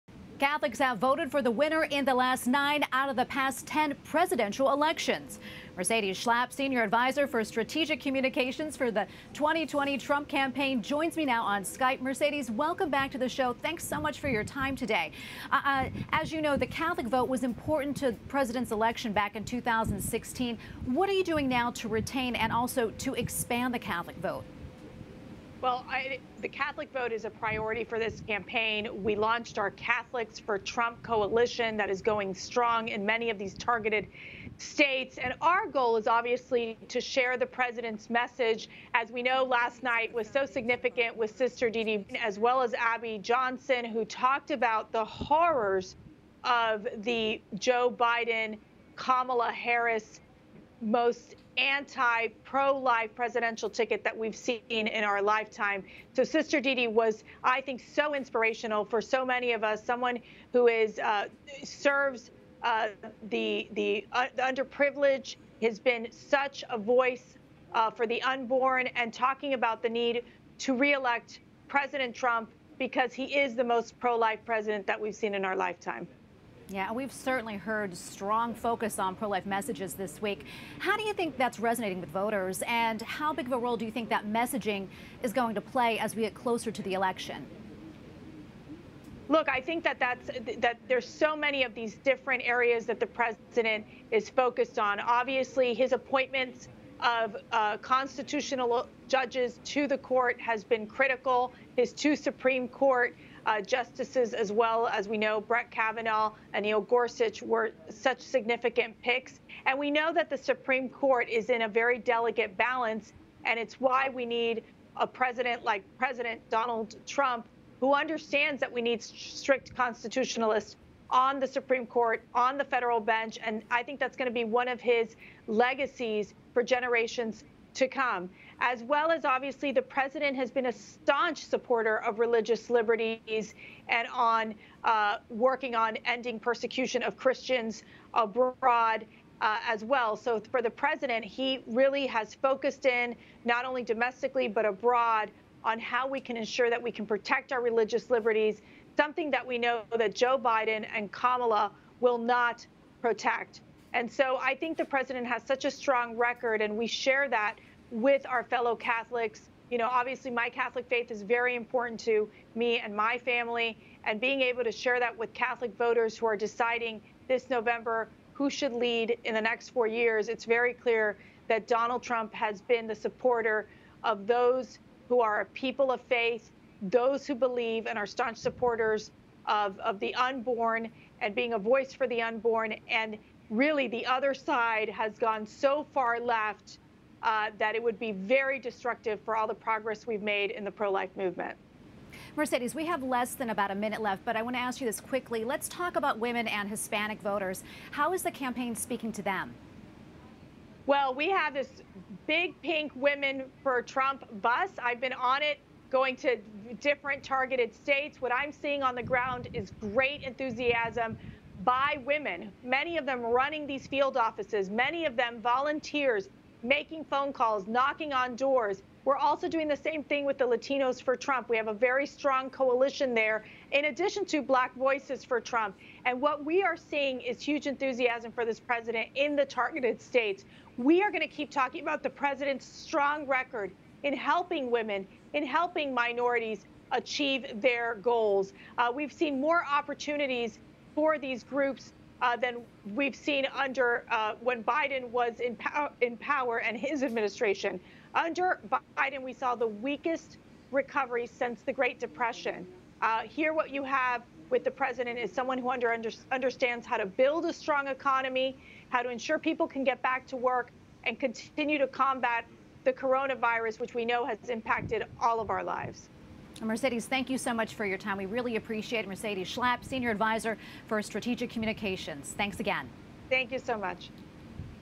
Catholic vote, Pro-life messaging: Mercedes Schlapp discusses 2020 Trump Campaign | EWTN News
In 2016, President Donald Trump and Hillary Clinton split the total Catholic vote. Mercedes Schlapp, 2020 Trump Campaign Senior Advisor for Strategic Communications, shares what the campaign is doing this time to reach Catholic voters. She also breaks down the strong pro-life messaging throughout the 2020 Republican National Convention.